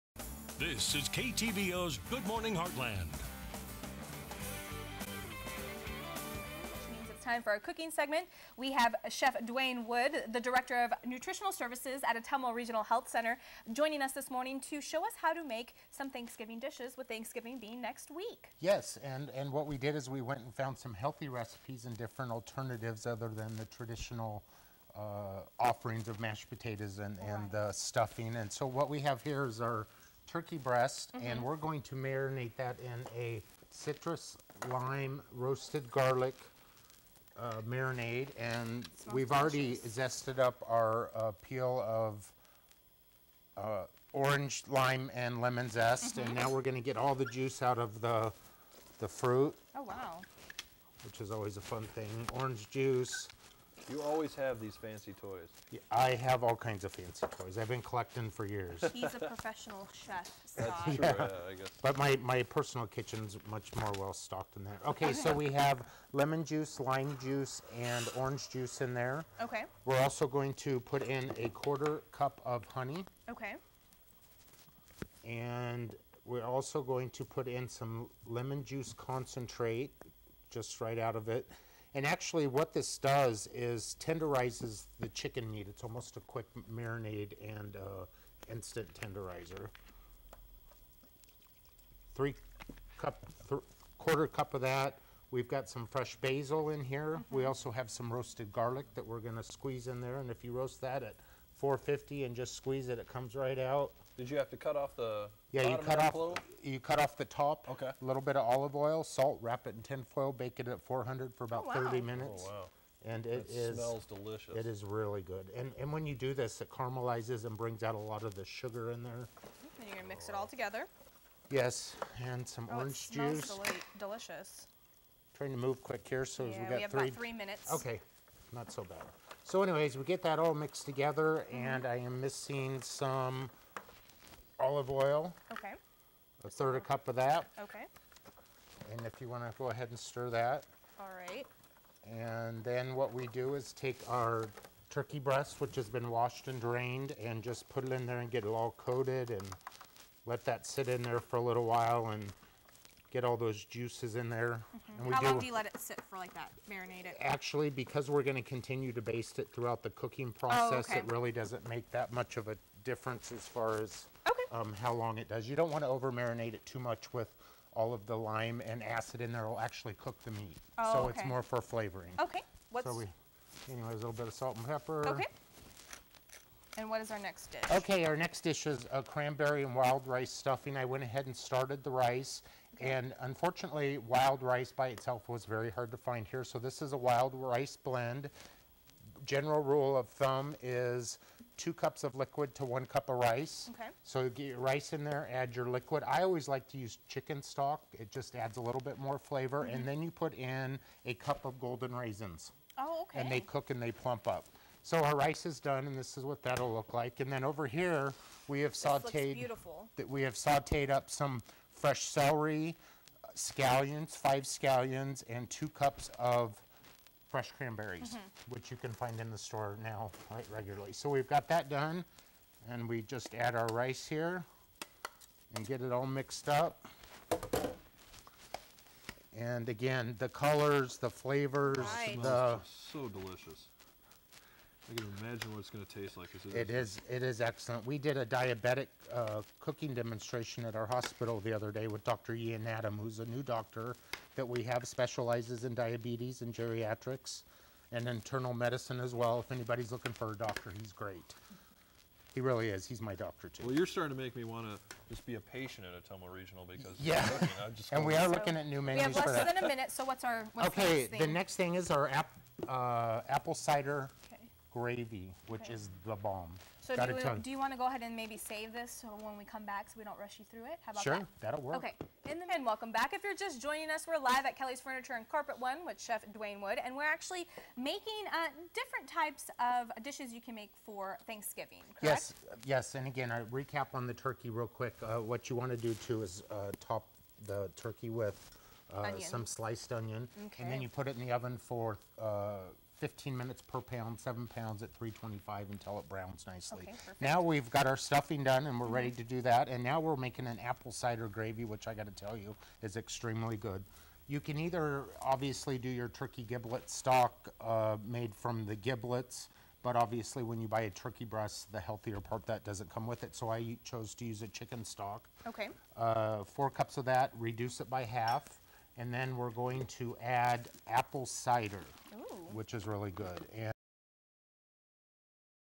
Alternative Thanksgiving Dishes
Chef Duane Woods shows us 3 different Thanksgiving dishes